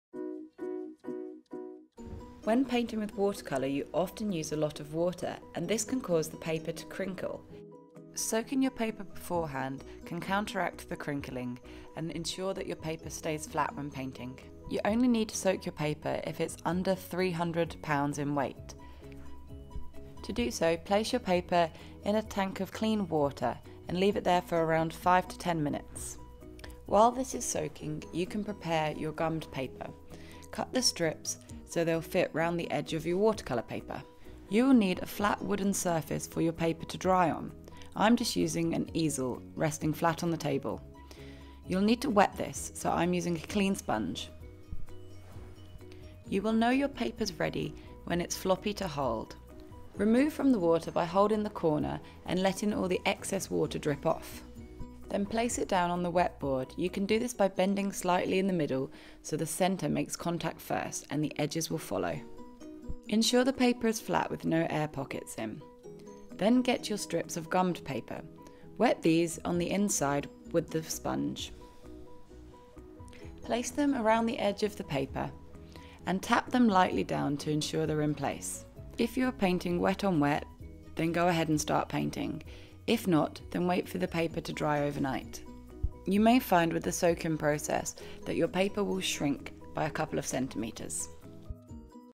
How to soak watercolour paper - Zieler
Instructional art help video showing you how to prepare your watercolour paper ready for painting. For the written guide to this video

This video breaks down the process of soaking your watercolour paper. This helps to prepare the paper for absorbing substantial amounts of water, ready for painting. The Zieler watercolour paper is 300gsm so is perfect for absorbing plenty of water

You will need a clean dry surface for your paper to dry on. We suggest using a flat table easel like this one

Other things you will need include:
scissors
gummed paper
tank of water
watercolour paper
sponge

Follow us for more creative art videos to inspire and guide you into the creative world.